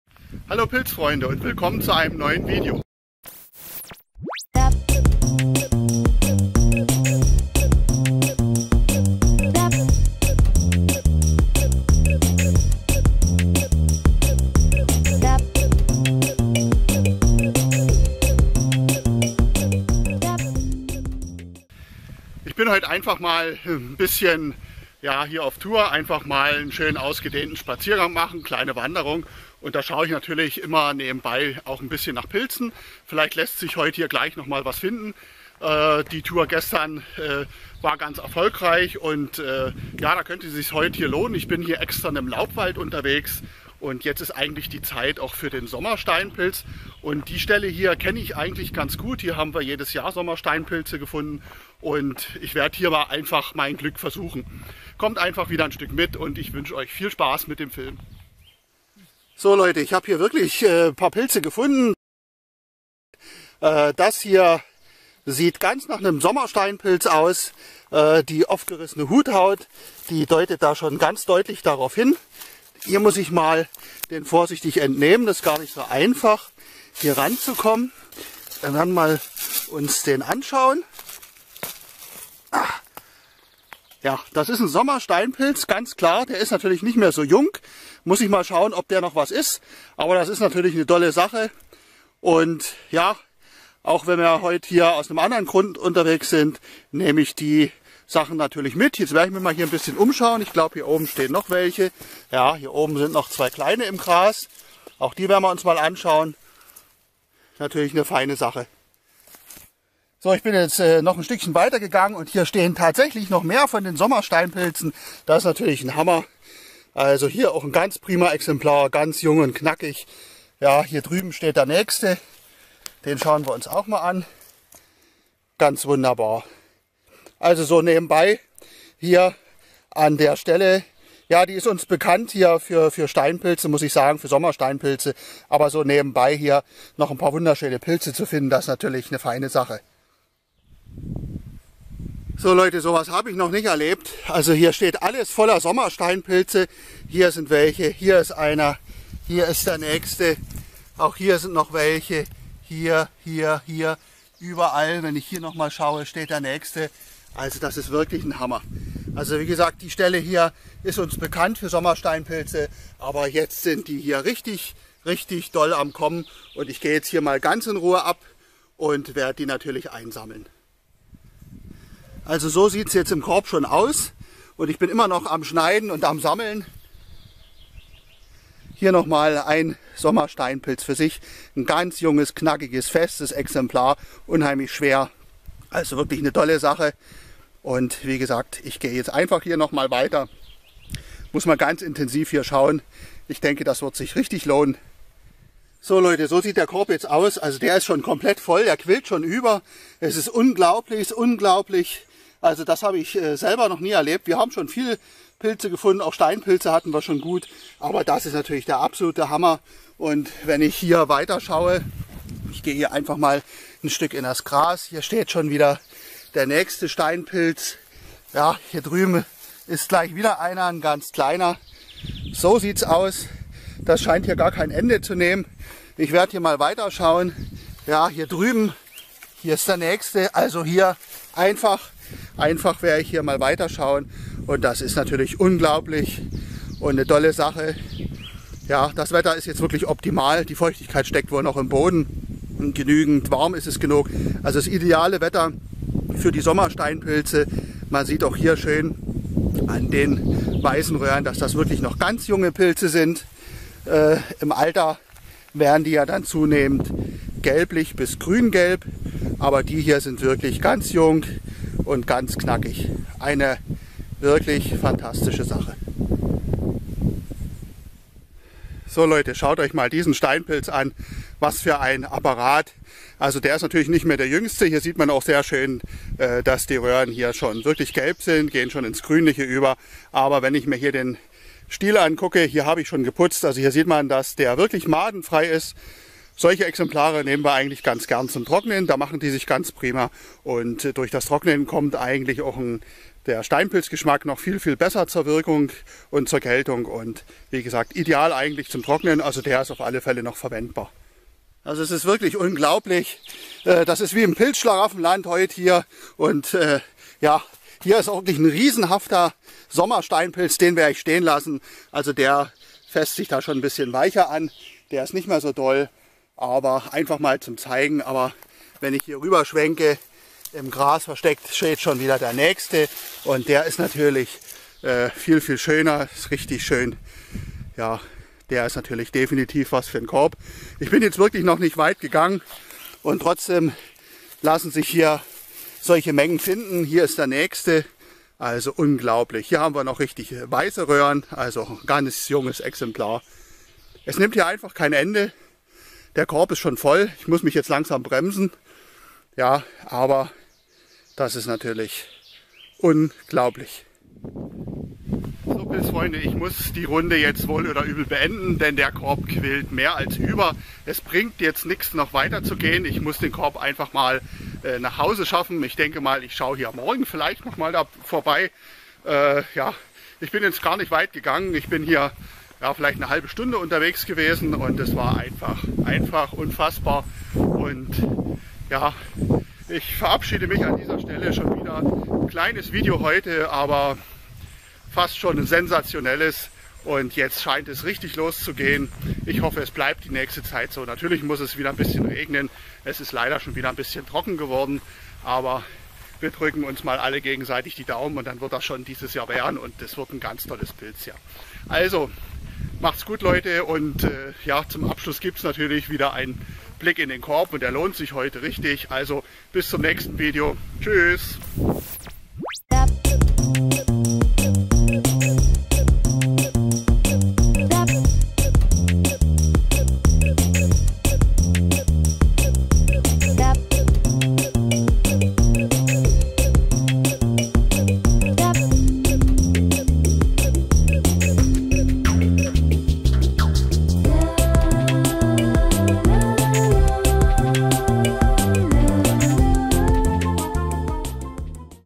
Sensationelle Sommersteinpilze
Hallo Pilzfreunde, eigentlich war nur eine kleine Wanderung angedacht. Zum Glück hatte ich den Korb dabei denn es ließen sich sensationelle Sommersteinpilze finden. So musste ich die Runde abbrechen und den vollen Korb nach Hause bringen.